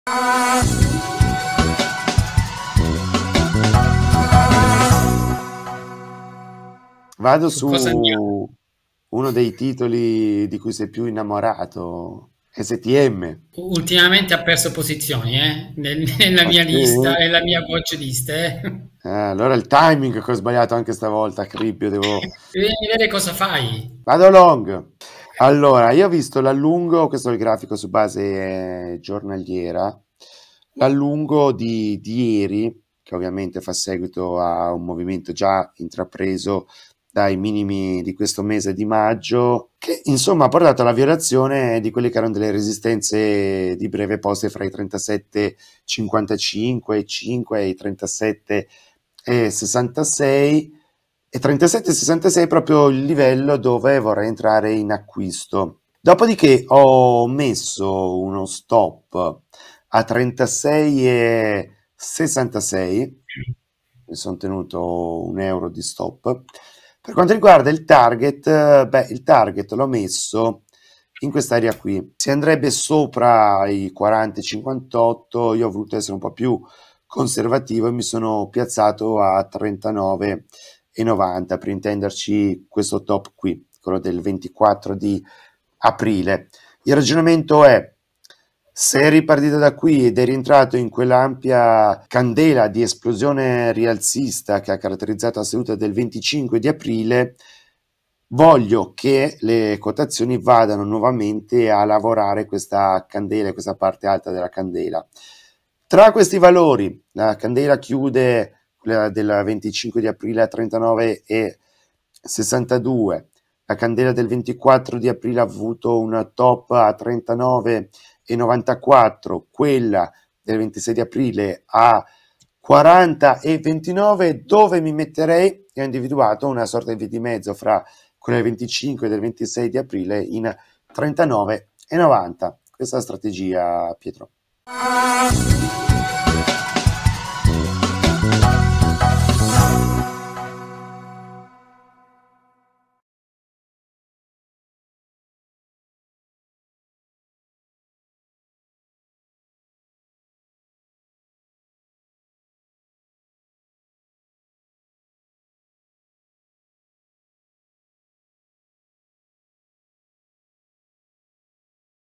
Investv: la ripartenza di STM potrebbe non essere finita, Designori prova il LONG
Nelle ultime settimane il titolo STM sembra aver posto le basi per una ripartenza dopo i ribassi che ne hanno caratterizzato l'andamento borsistico da metà dicembre 2023 in avanti. Questa l'idea sottostante alla strategia rialzista proposta in occasione della puntata dell'8 maggio 2024 di Investv da Riccardo Designori. Il rappresentante della squadra dei capitani vorrebbe sfruttare un eventuale ritracciamento dopo i rialzi di ieri per attivare la strategia multiday sul titolo quotato alla Borsa di Milano.

👉 Guarda il video e scopri i livelli operativi indicati durante la diretta di Investv